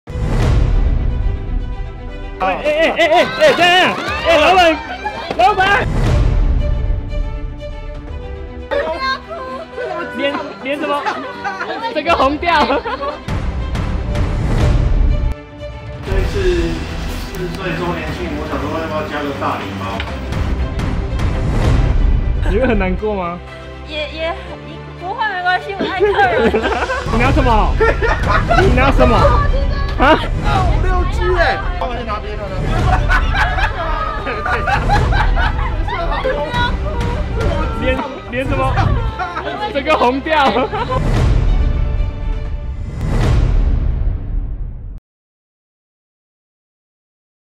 【HerherS四歲生日派對】老闆終於發瘋了！發瘋後竟然爆哭！？
絕對最狂生日派對週年慶！